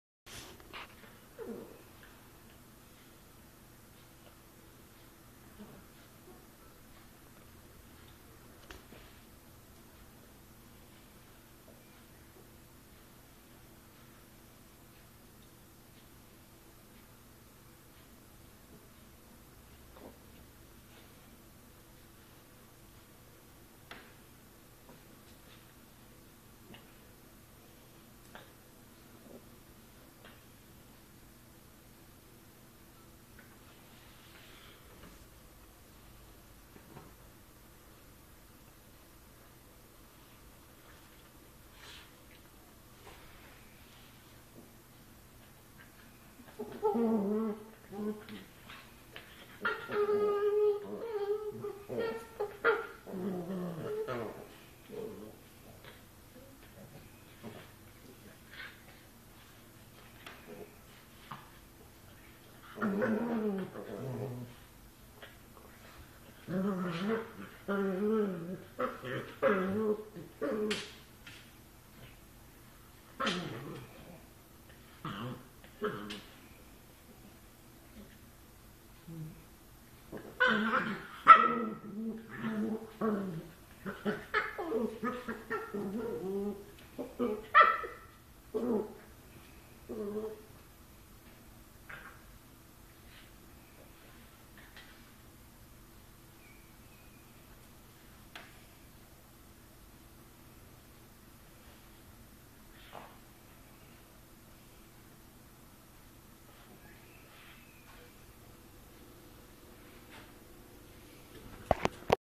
Play Fighting.mp4
Holly's trying to chew Jasper's foot